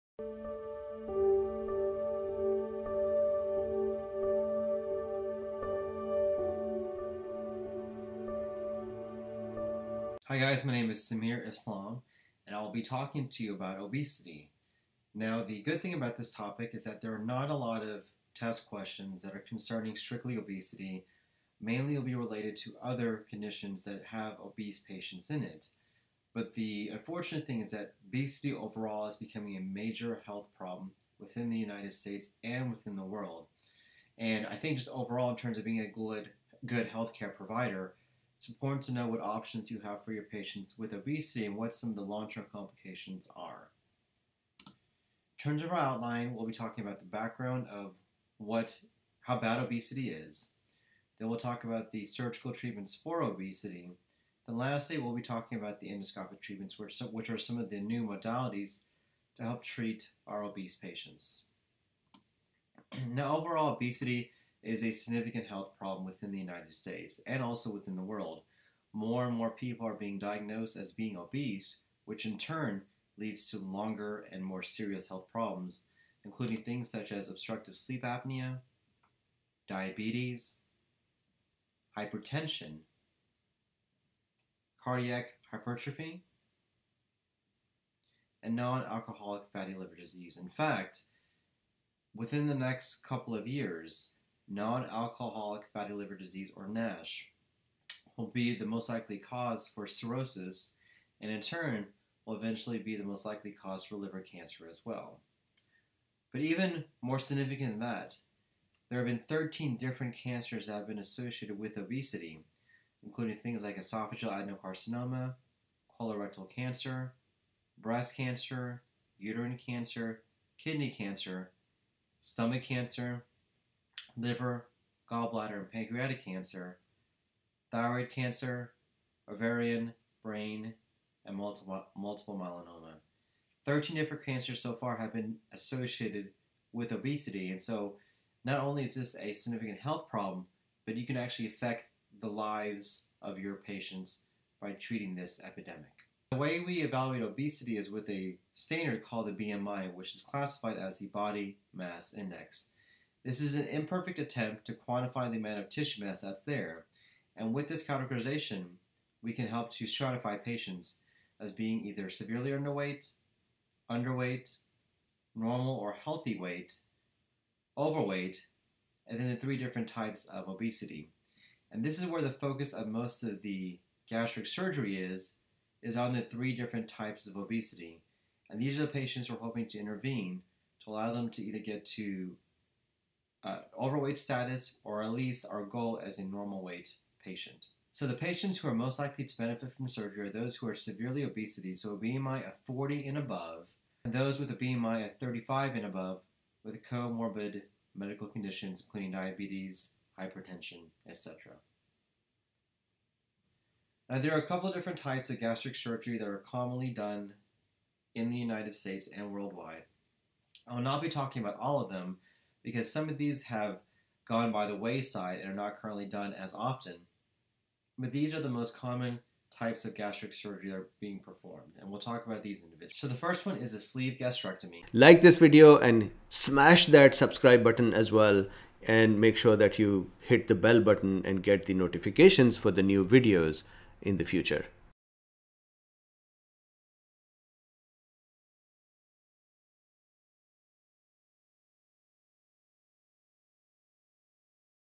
Obesity an Introduction and Management Approaches
. Obesity is a significant health problem causing adverse effects such as obstructive sleep apnea, diabetes, hypertension, cardiac hypertrophy, non-alcoholic fatty liver disease, and 13 different cancers.

In this video, Dr. Sameer Islam effectively describes the evaluation tool for obesity i.e BMI. He also discusses possible treatment approaches.

*Adverse effects of obesity.
*Evaluation of obesity.
*Surgical treatment approaches of obesity.
*Types of gastric surgery.
*Endoscopic treatment approaches of obesity. ...